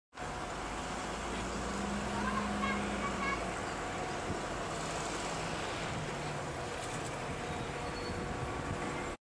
Halo Phenomenon
This short video show the halo phenomenon that occured in Brunei noon time of Apr 8, 2014. It was happened several times but this period attracted to the people which lot of sharing through social networks.